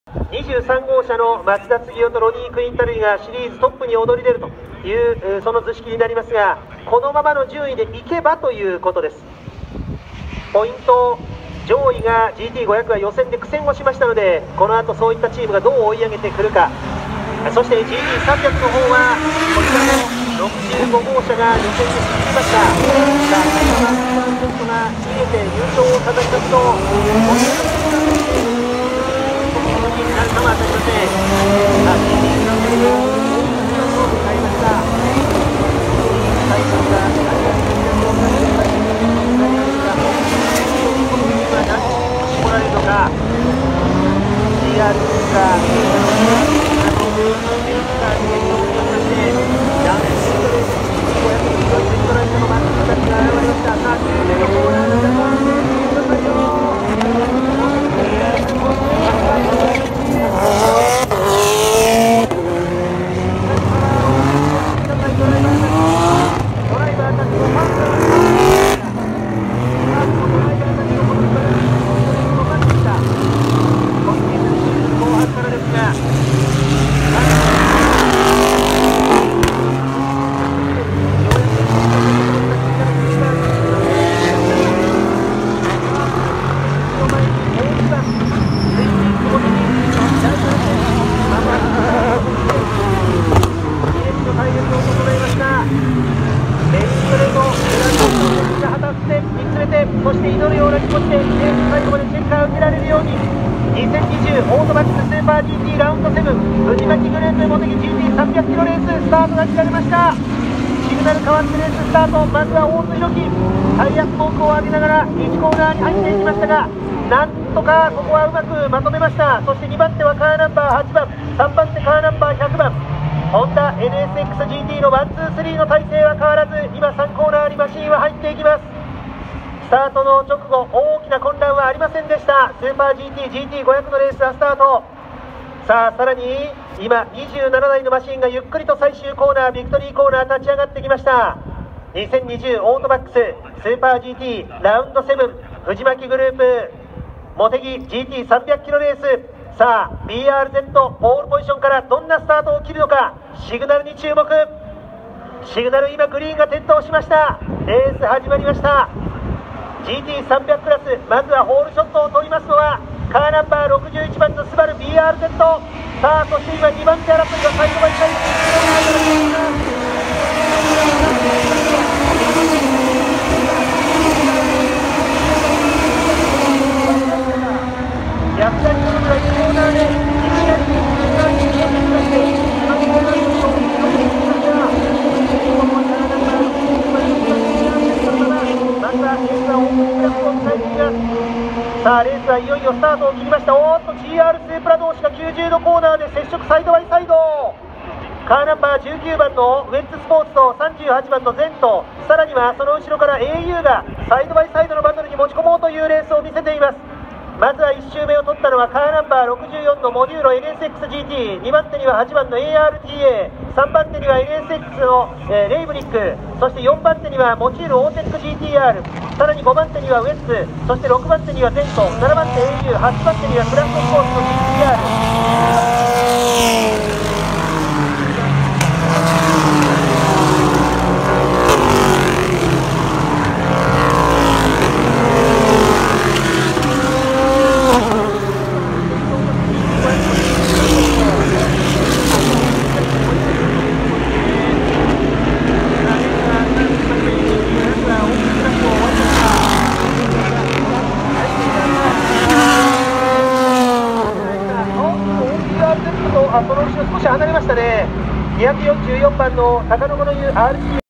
④ 2020 SUPER GT Rd.7 MOTEGI GT 300km RACE フォーメーションラップ→スタート，オープニングラップ
11月8日　V字コーナー立ち上がり